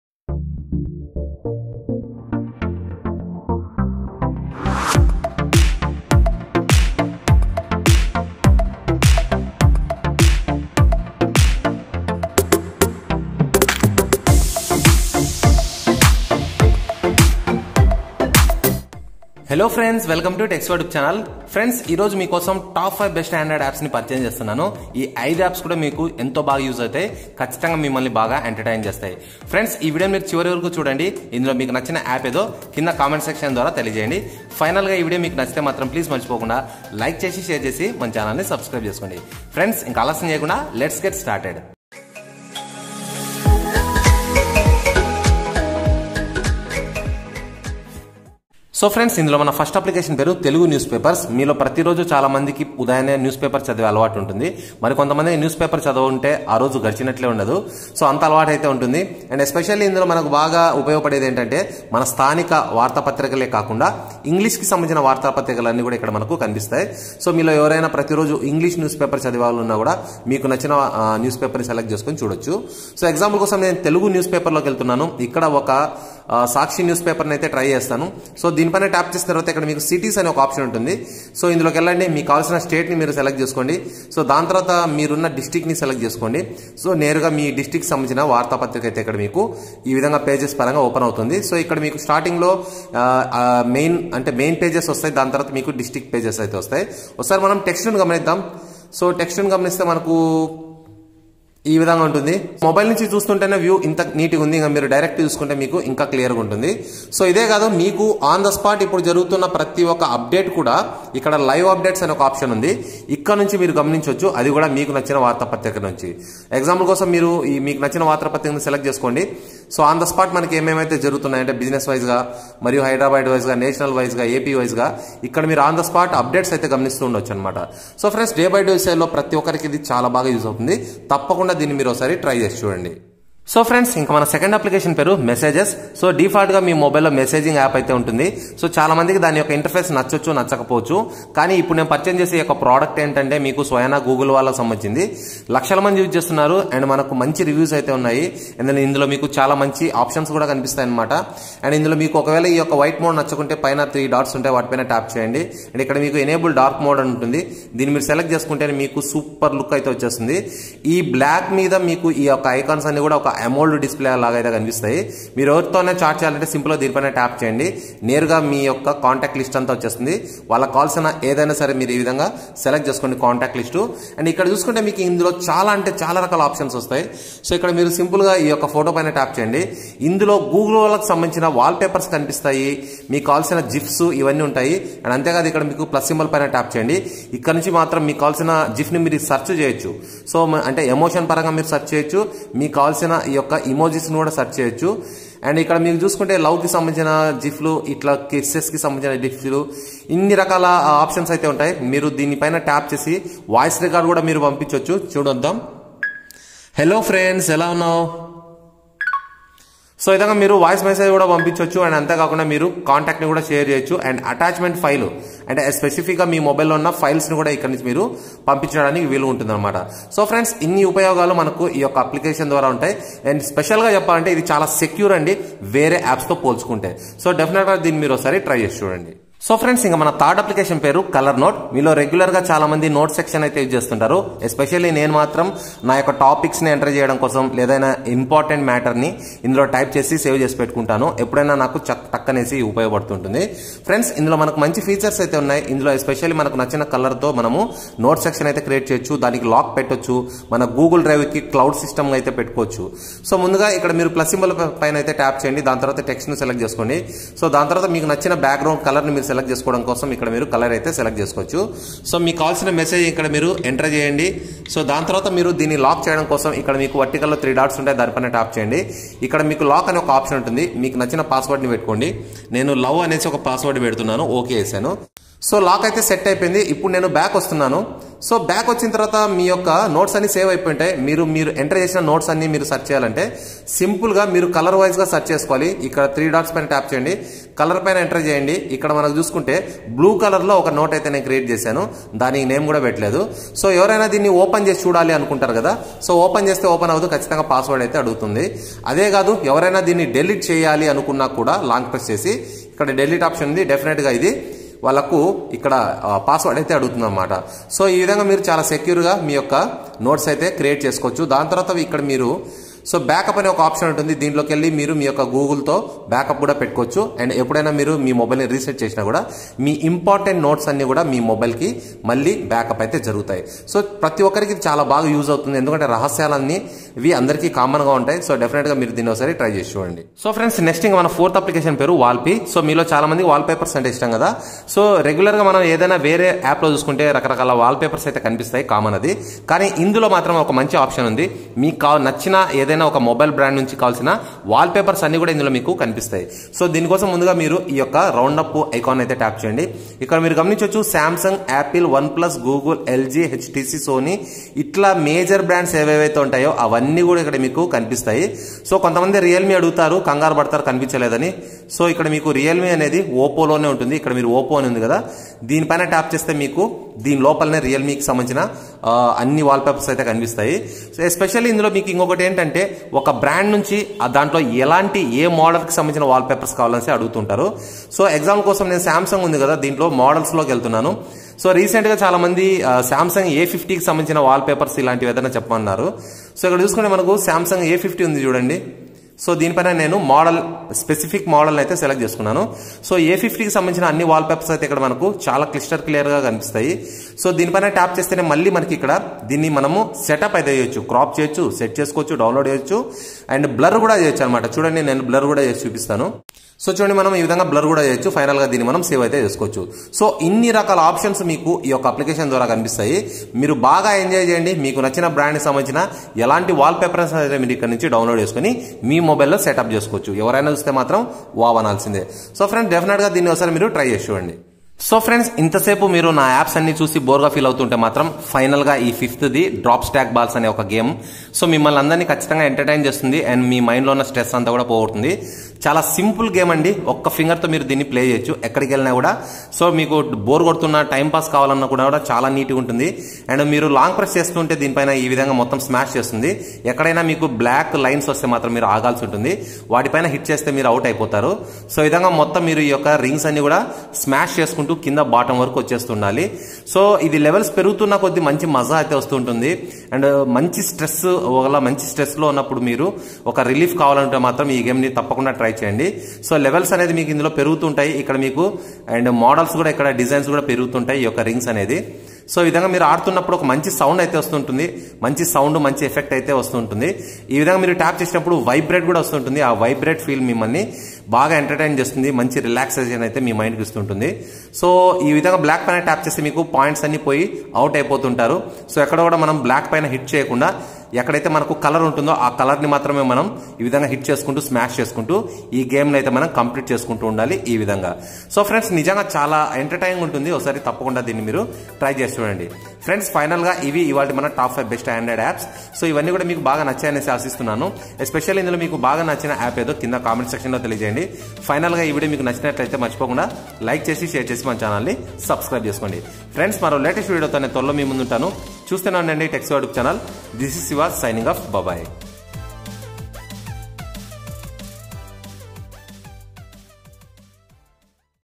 Top 5 Best Android Applications 2019 | Latest Stress Relieving Game App For Android | Tech Siva
Top 5 Best Android Applications 2019. Today, I present you the Latest Stress Relieving Game and Other best applications for your Android mobiles.